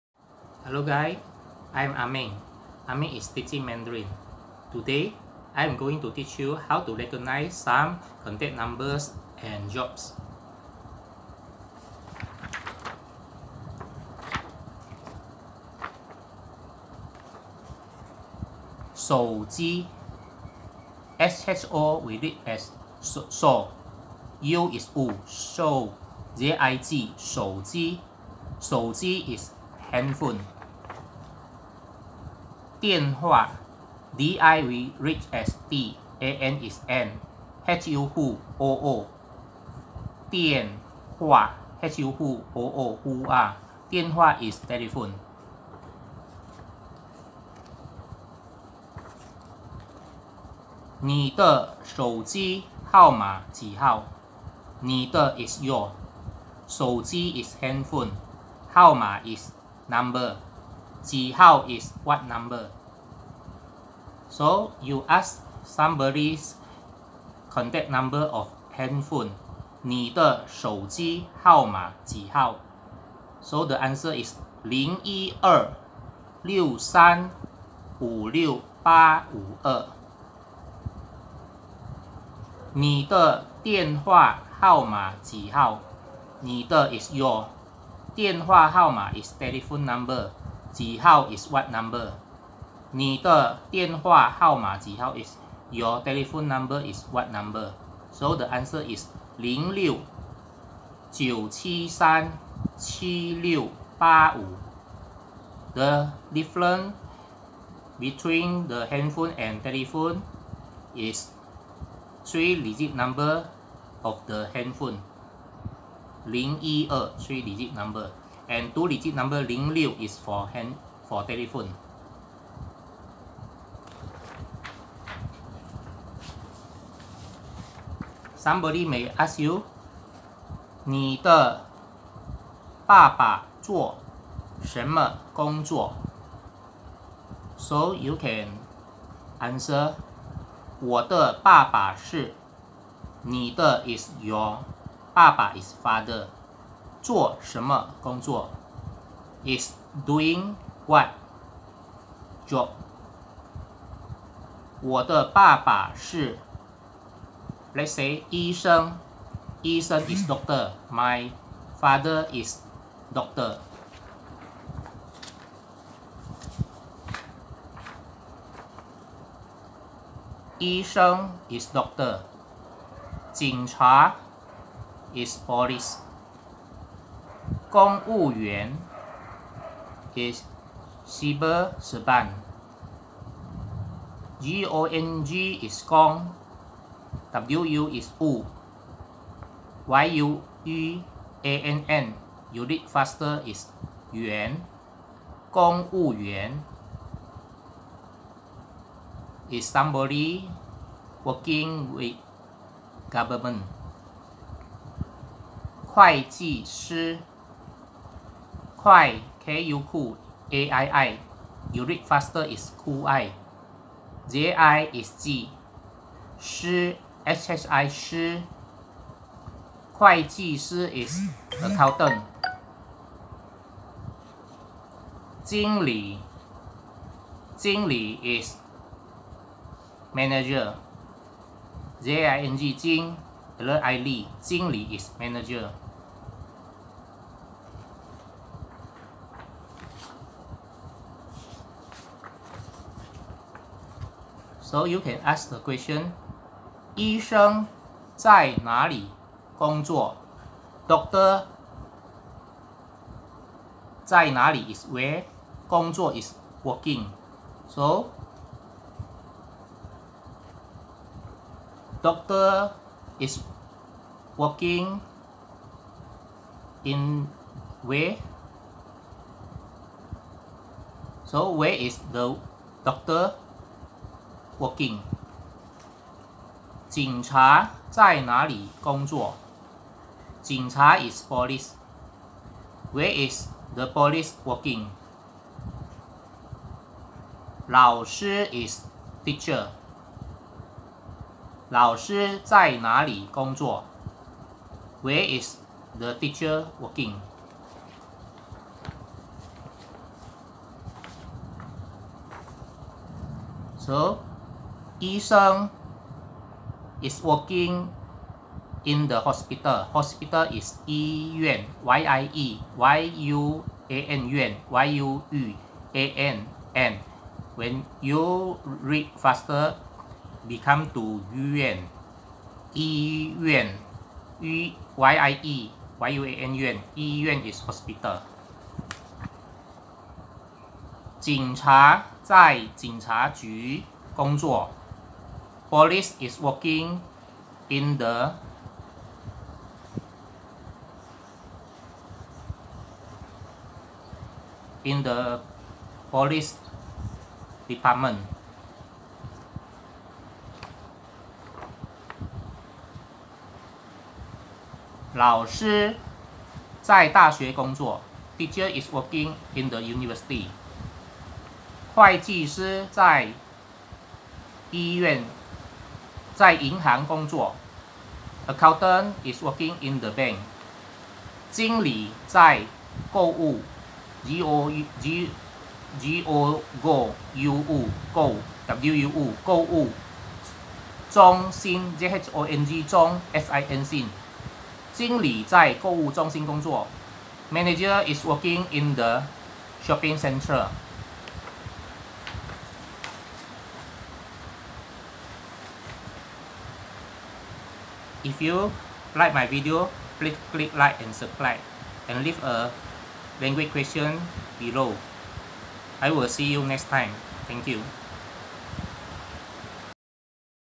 Mandarin Learning:How to Recognise Some Contact Numbers And Common Jobs?
I am Ah Meng. Ah Meng is teaching Mandarin.